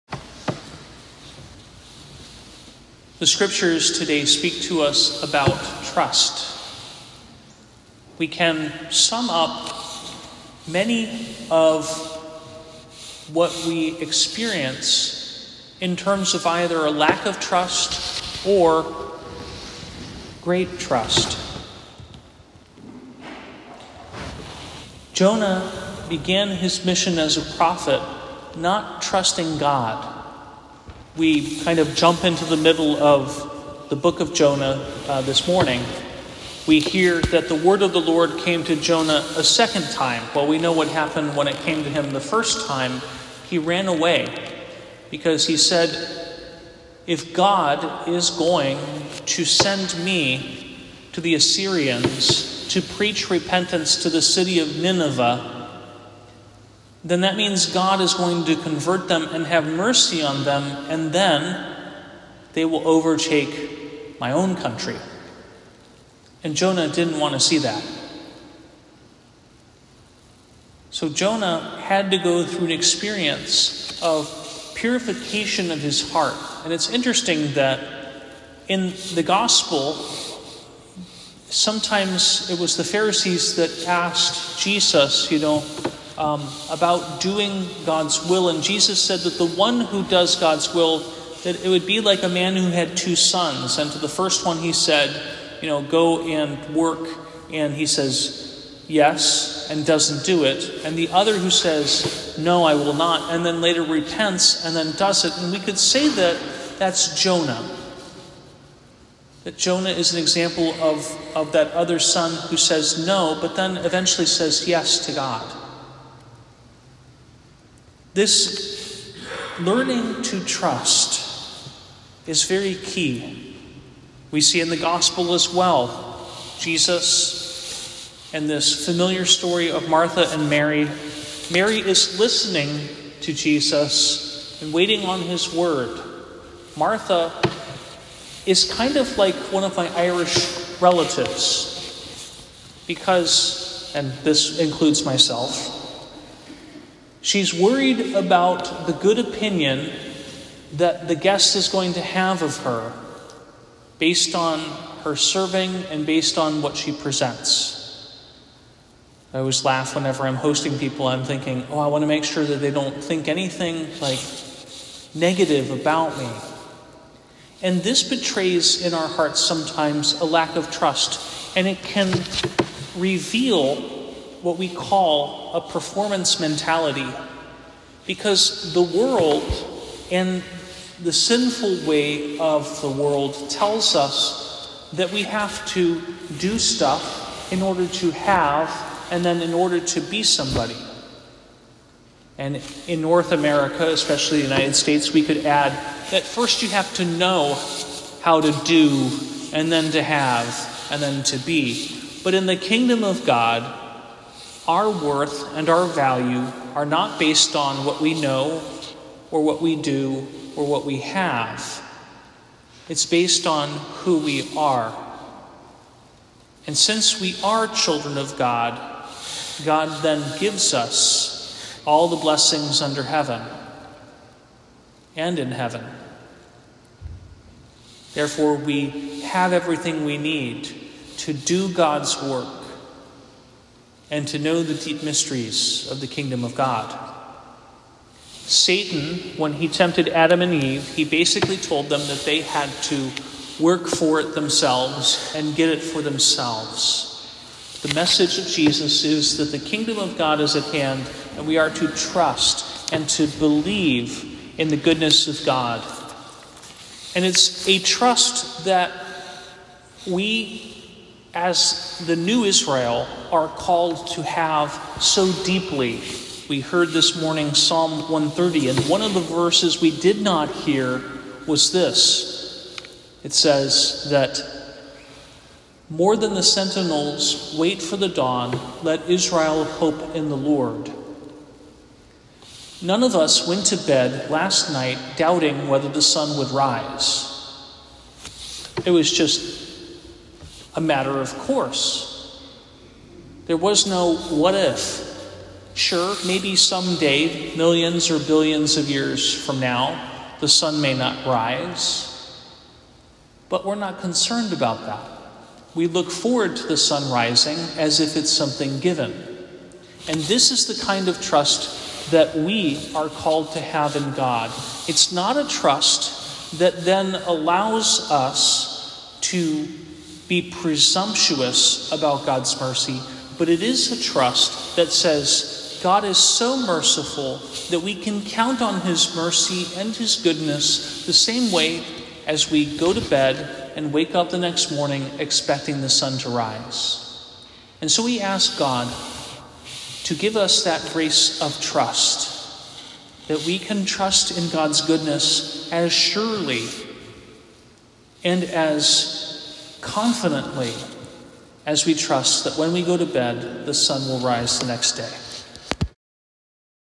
October 5, 2021
Homily for Tuesday of the 27th Week in Ordinary Time Year I
Jon 3:1-10, Ps 130, Lk 10:38-42